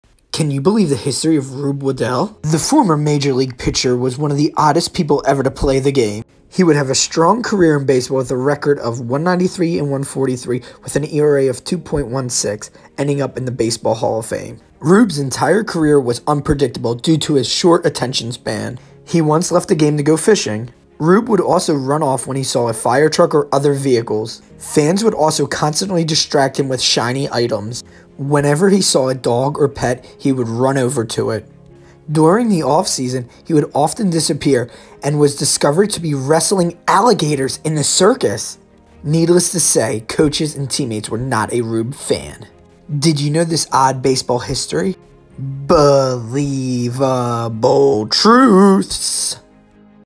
Rube Waddell, the oddest man in baseball history!
Can you believe the odd baseball history of Rube Waddell? Former MajorLeagueBaseball Pitcher was one of the oddest people ever to play the game.